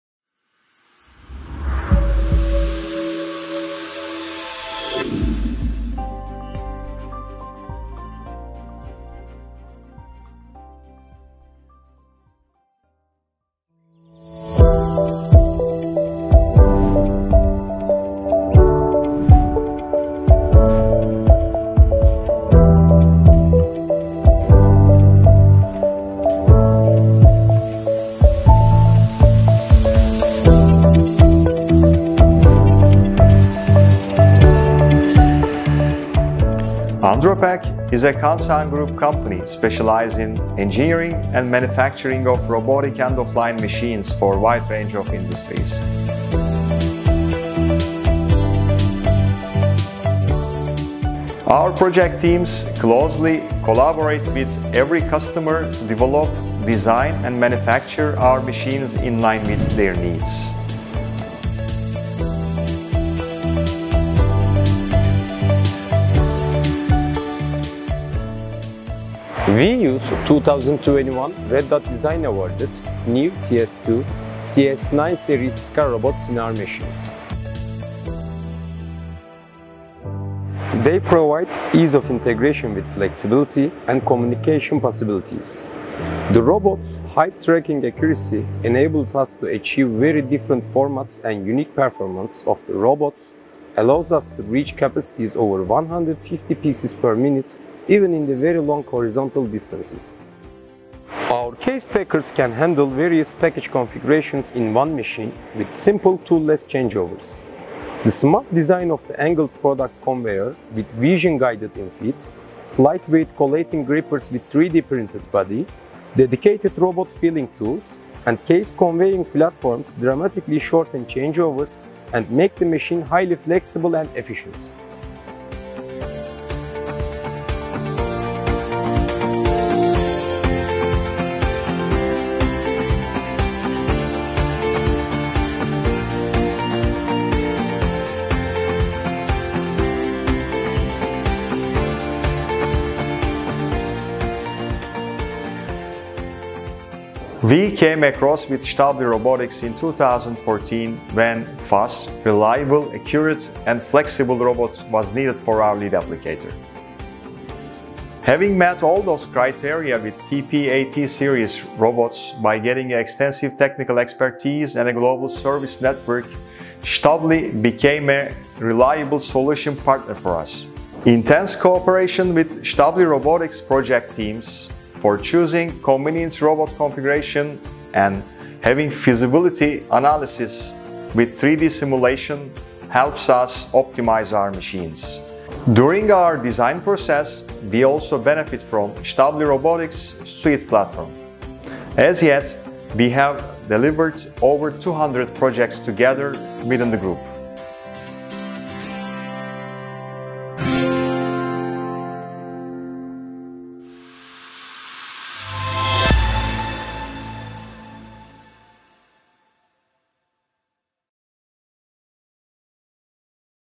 Robotics Customer Voices: Andropack
Andropack based in Turkey, is a Kansan Group company specialized in engineering and manufacturing of robotic end-of-line machines for a wide range of industries.

Stäubli Robotics and Andropack have a close collaboration in order to design and develop case packers handling various package configurations in one machine with simple & tool-less changeovers with the use of SCARA TS2 robots.